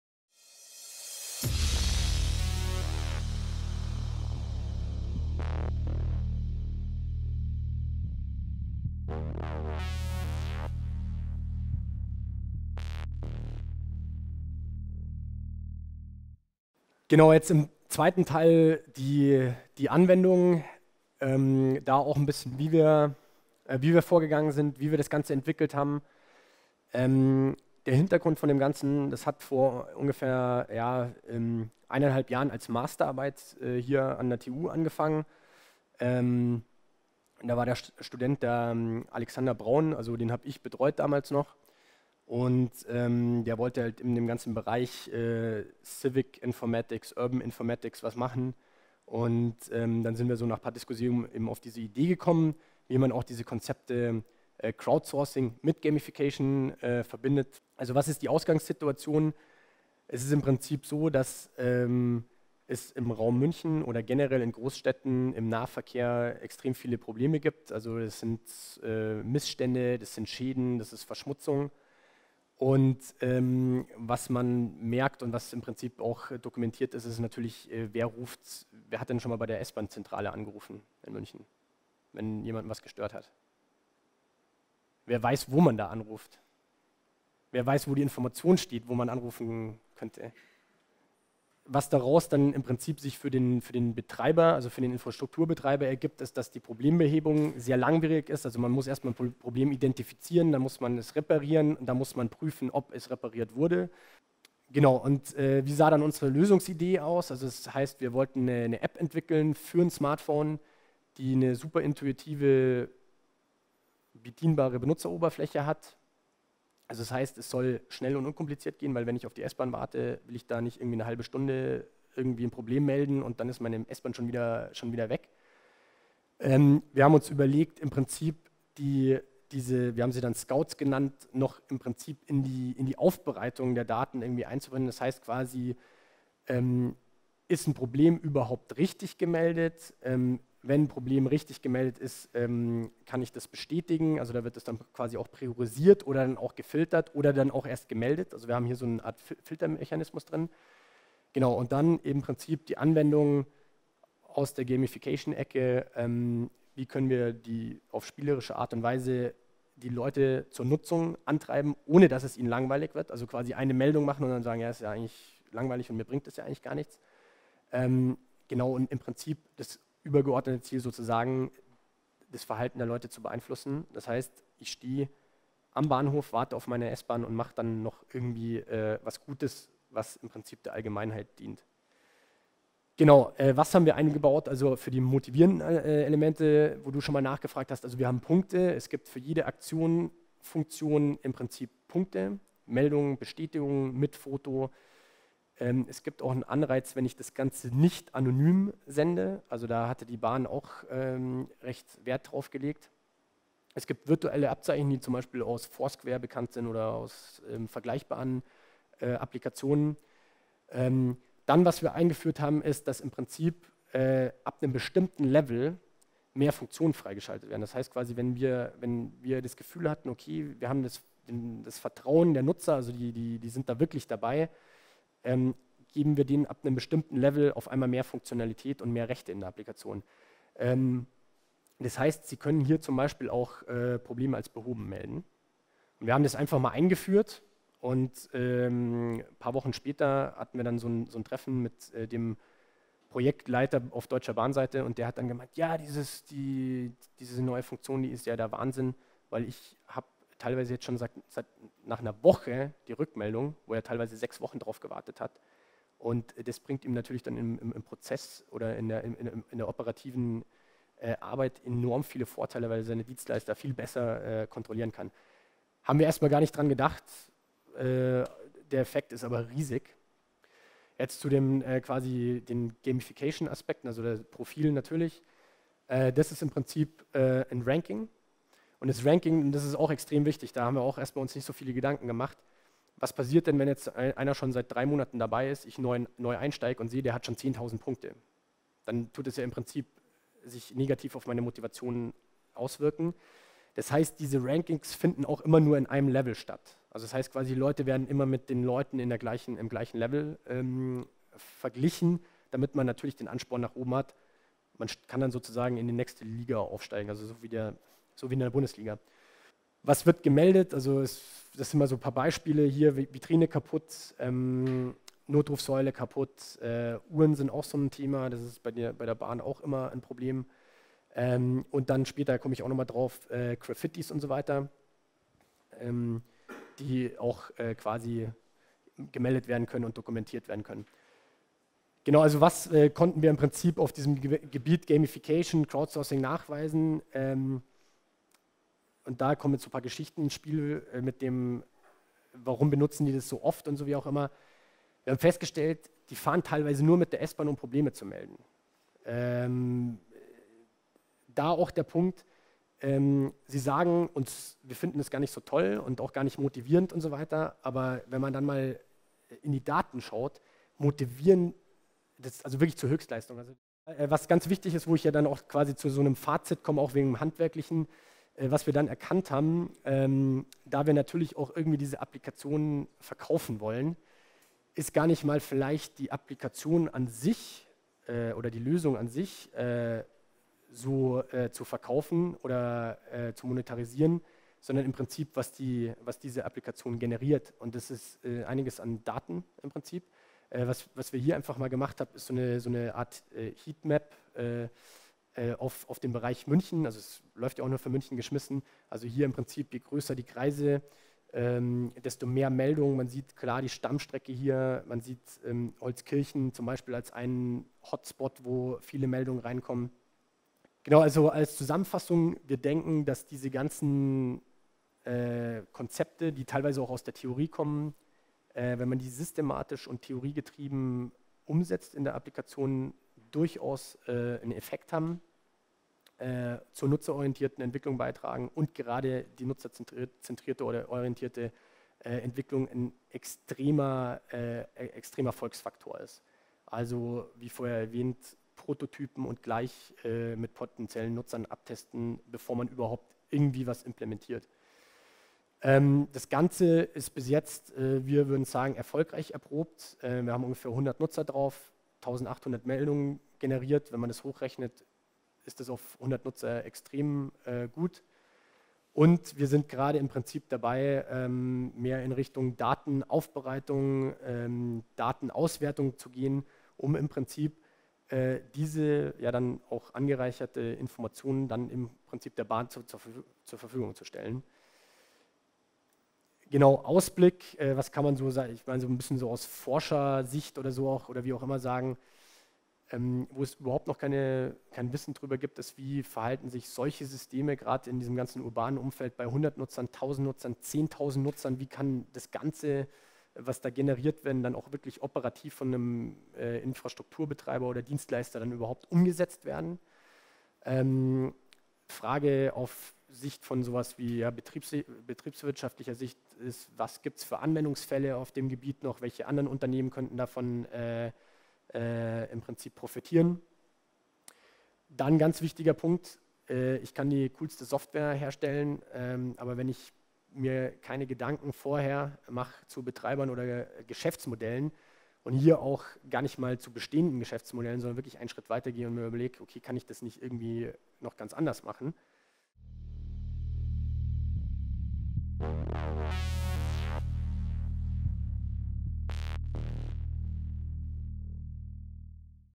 Felix Köbler - BahnScout - IF-Akademie (Teil 2)
"BahnScout" - der zweite Teil des Vortrages von Felix Köbler bei der IF-Akademie  am Donnerstag, den 18.Juli. "BahnScout" steht für eine Anwendung mit einer APP, die für saubere und funktionierende Bahnhöfe sorgt. Das Video wurde anlässlich seines Vortrages für die InterFace AG im Saal Konrad Zuse der TUM in Garching erstellt.